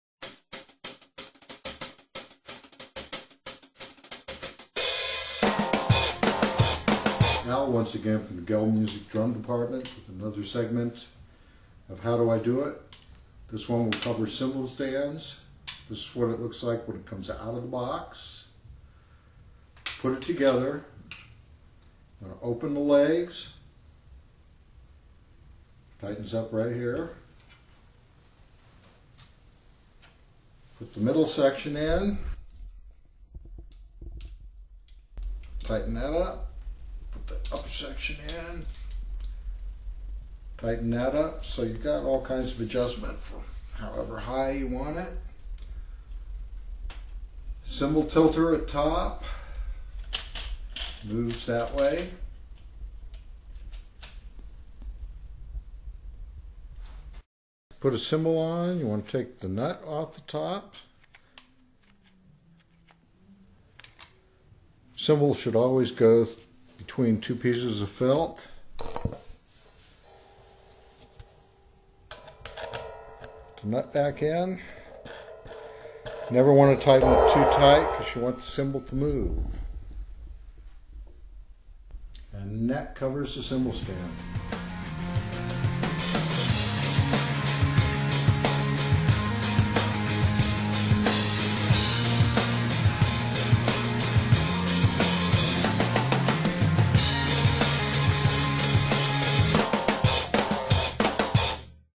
How to Set Up Your Cymbal Stand. - Gelb Music Drum Education
Gelb Music drum educational video series continues with this installment of "How do you do that?". This video shows you how to set up your cymbal stand from out of the box.